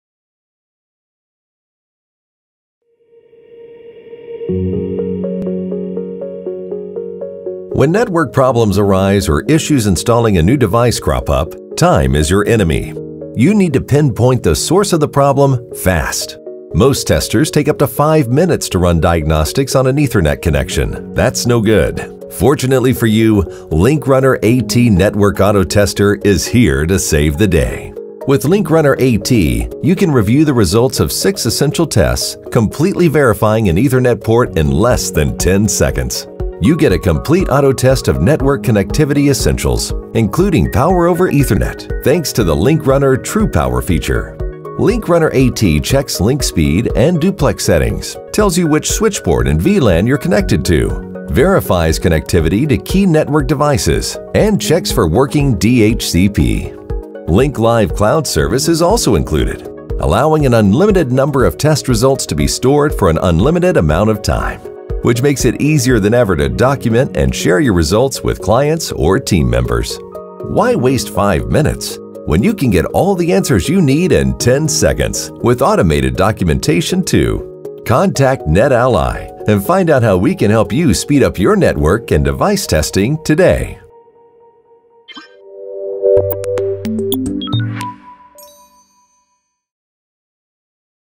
NetAlly LinkRunner AT Network Auto-Tester
The LinkRunner AT Network AutoTester offers user-configured AutoTests for a wide range of important tasks. This network tester’s quick cable test and switch identification facilitate fast problem isolation.

Test results can be automatically uploaded to the Link-Live Cloud Service to improve collaboration between network engineers and technicians, creating greater job visibility, project control, and fleet management.

Join the Comms Community!